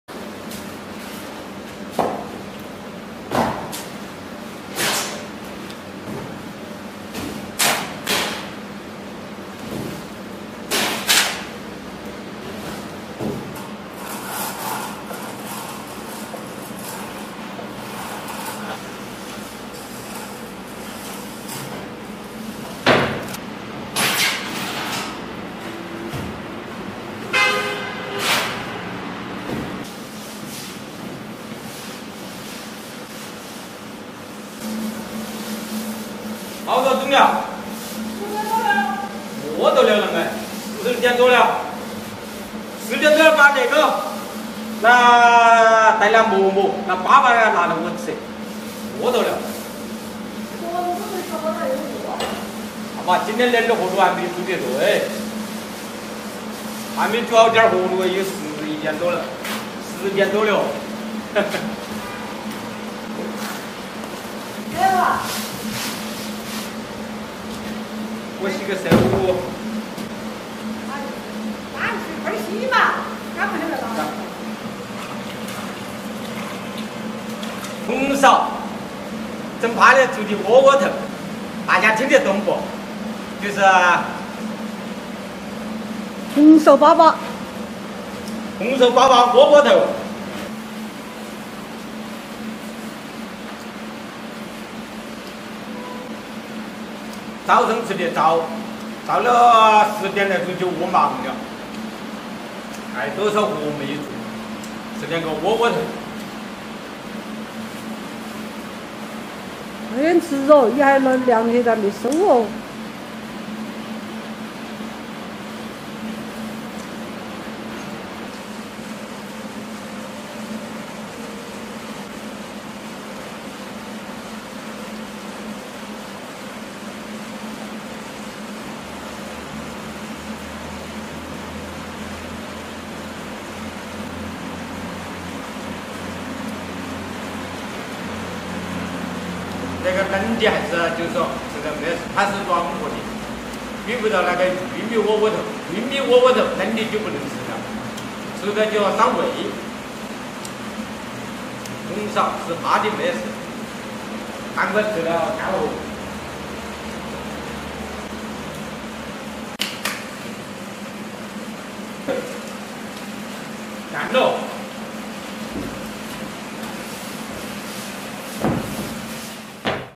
农民工肚子太饿了，10点钟吃2个窝窝头充饥，你吃过这种没？
农民工 工地 外卖 美食 夫妻 请注意，本视频完全符合YouTube社区准则，没有任何违规行为。